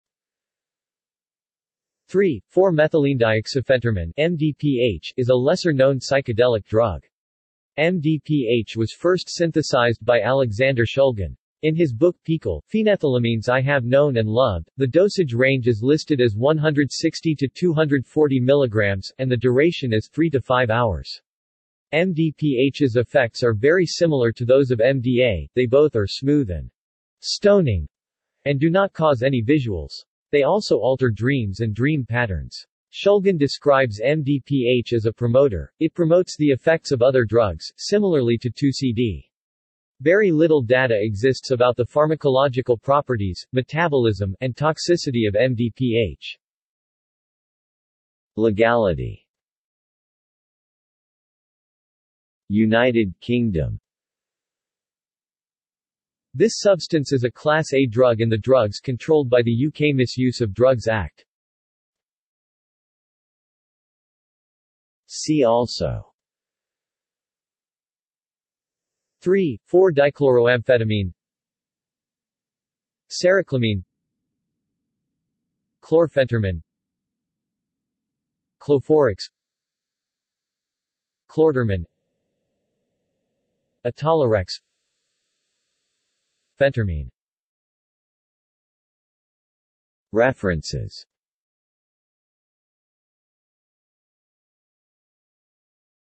3,4-Methylenedioxyphentermine | Wikipedia audio article
This is an audio version of the Wikipedia Article:
3,4-Methylenedioxyphentermine

SUMMARY
3,4-Methylenedioxyphentermine (MDPH) is a lesser-known psychedelic drug. MDPH was first synthesized by Alexander Shulgin. In his book PiHKAL (Phenethylamines i Have Known And Loved), the dosage range is listed as 160–240 mg, and the duration as 3–5 hours. MDPH's effects are very similar to those of MDA: they both are smooth and "stoning," and do not cause any visuals.  They also alter dreams and dream patterns.  Shulgin describes MDPH as a promoter; it promotes the effects of other drugs, similarly to 2C-D. Very little data exists about the pharmacological properties, metabolism, and toxicity of MDPH.